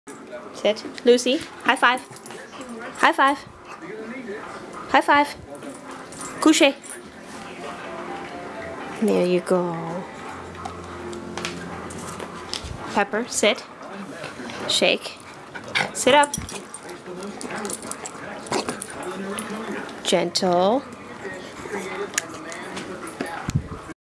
Lucy and Pepper do tricks, lame ones.
Lucy the boxer and Pepperoni the dachshund do their tricks.  I'll get a better video of this later, just trying out the new camera.